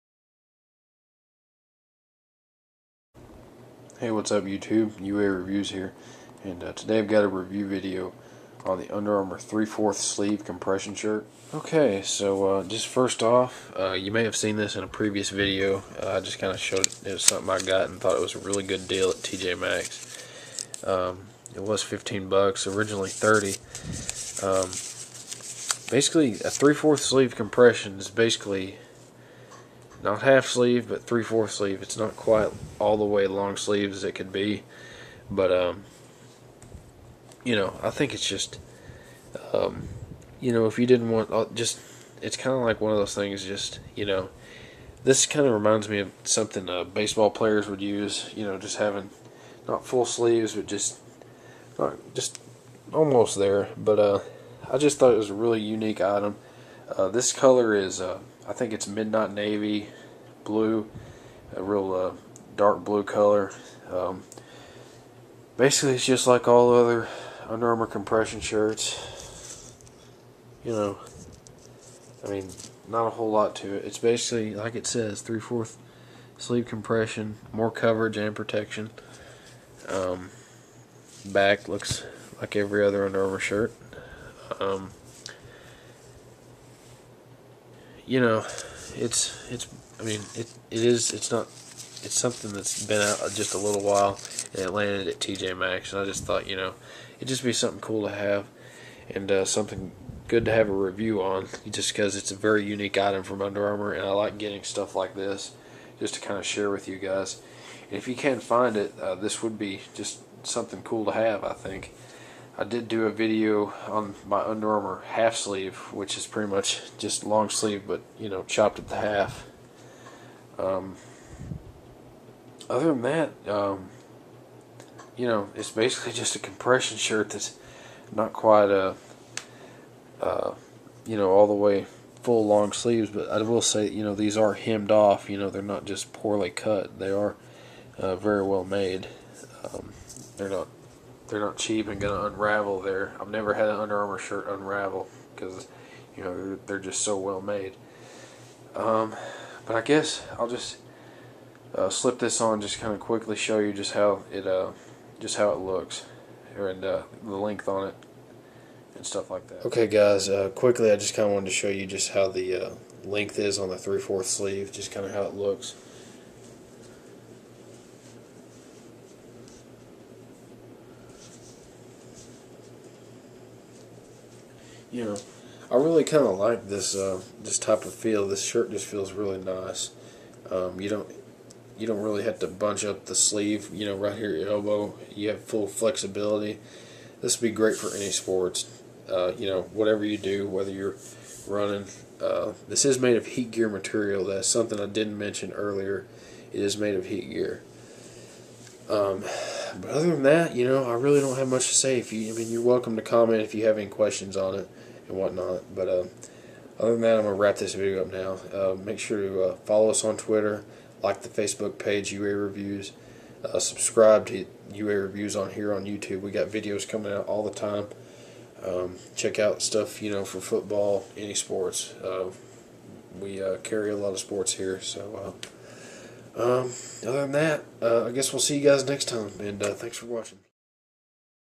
Under Armour 3/4 Sleeve Compression T Review
** One of my newest Under Armour Compression Shirts, TJ Maxx had this Under Armour 3/4 Sleeve Compression T,